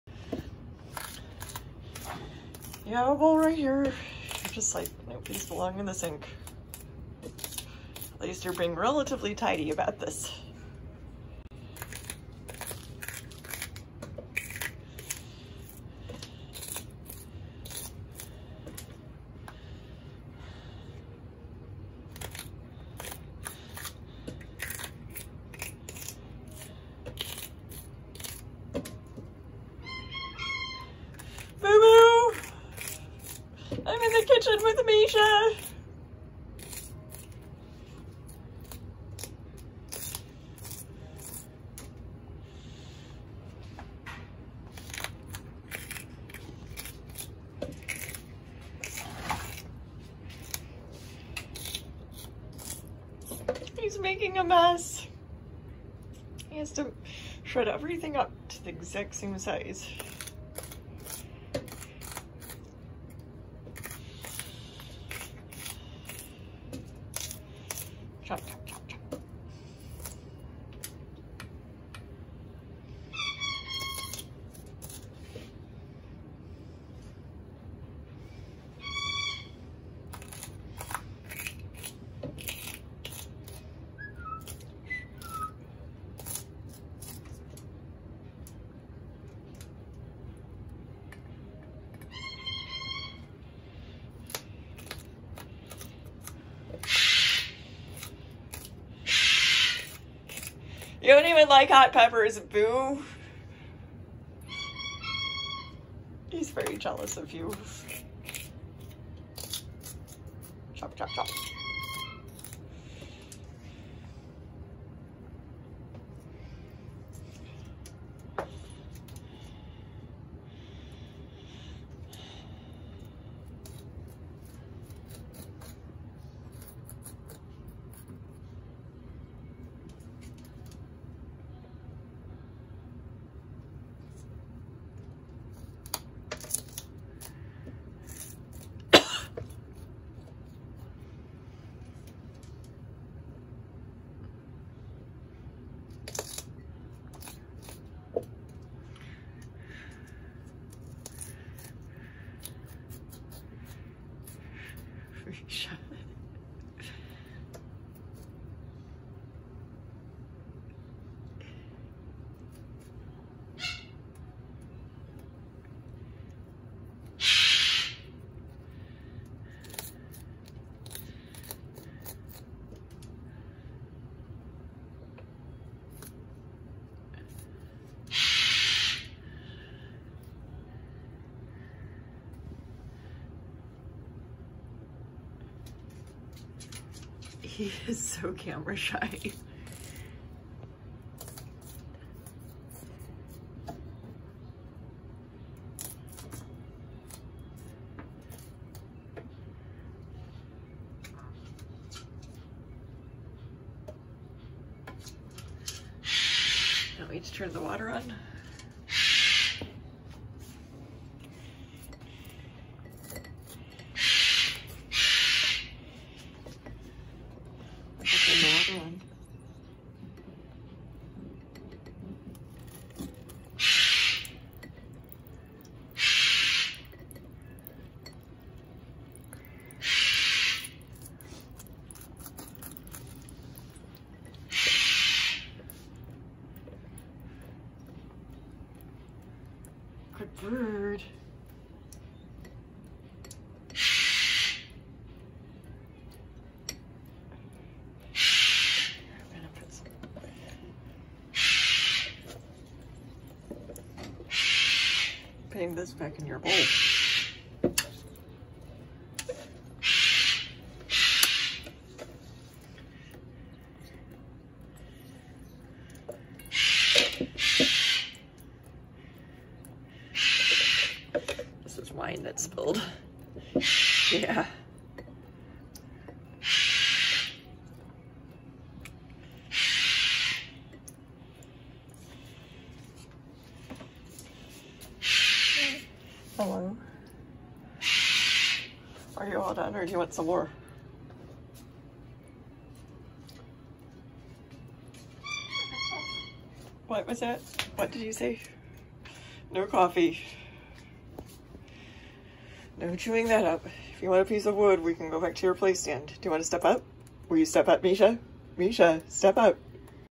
Misha the Moluccan cockatoo eats jalapeno peppers for breakfast!
Misha the Moluccan cockatoo is making a big mess of hot jalapeno peppers in my sink! I've given him a whole package and he crunches and shreds each an every one. It is so hard to keep a clean house with such messy birds- I'd appreciate any tips! Since I've been staying home more and my new guy has been coming over, the birbs get more attention but I'm also more aware of their mess an have to live with it more than I used to.